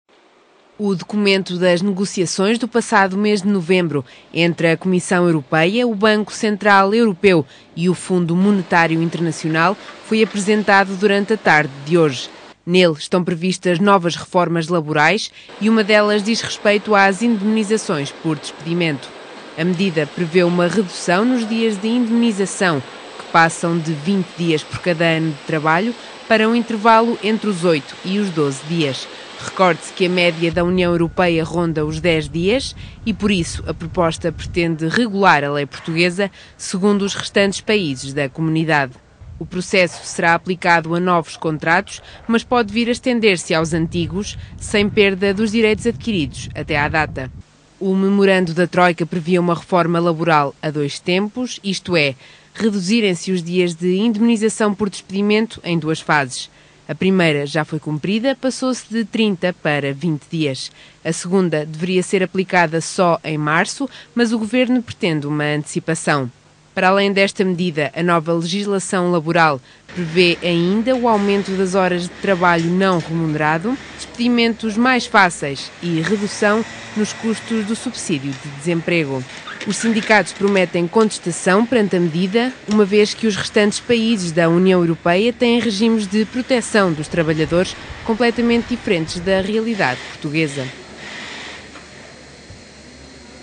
TROIKA - MEMORANDO PREVÊ MAIOR LIMITE NAS INDEMNIZAÇÕES
O Governo vai reduzir o número de dias de indemnizações por despedimento e pretende alinhar a realidade Portuguesa ao resto dos Países da União Europeia.